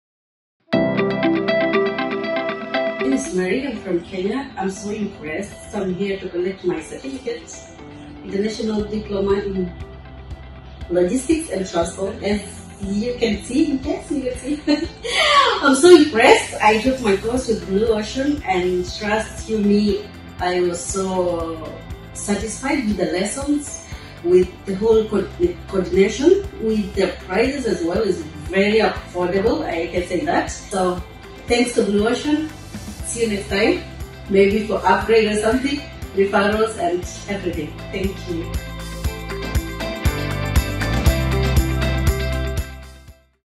Student Testimonial - CILT Level 5 - Mary Kalondu Mbithi
Your desire to climb up the ladder of career growth will be well supported by the CILT Level 5 course that you've completed with us.
Congratulations Mary Kalondu Mbithi on your achievement.